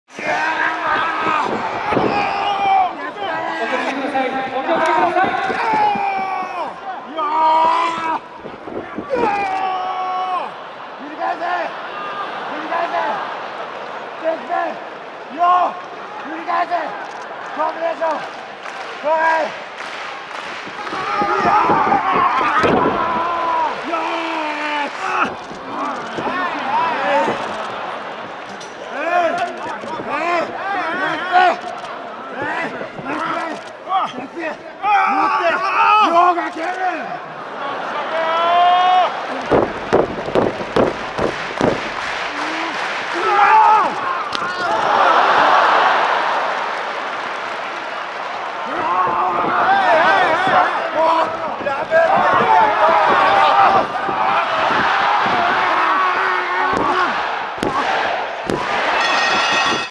【Fighting talk!】逸材はどんな時でも頑張る【新日本プロレス】
🔥リング上で繰り広げられる、アツき言葉の闘い🔥
新日本プロレスのYouTube shorts動画【Fighting talk!】シリーズ

2023.4.8 東京・両国国技館
CSテレ朝チャンネル Presents テレビ朝日 新日本プロレス放送50周年記念 SAKURA GENESIS 2023

新日ちゃんぴおん。グランプリ獲得記念特別試合
棚橋 弘至 & エル・デスペラード &  YOH
vs
鈴木 みのる & 矢野 通 & グレート-O-カーン

👆この試合を全部観たい方は新日本プロレスワールドで！！

🔰真壁先生による、誰にでもわかる新日本プロレス講座はコチラから🔰
プロレスのルール、楽しみ方を解説！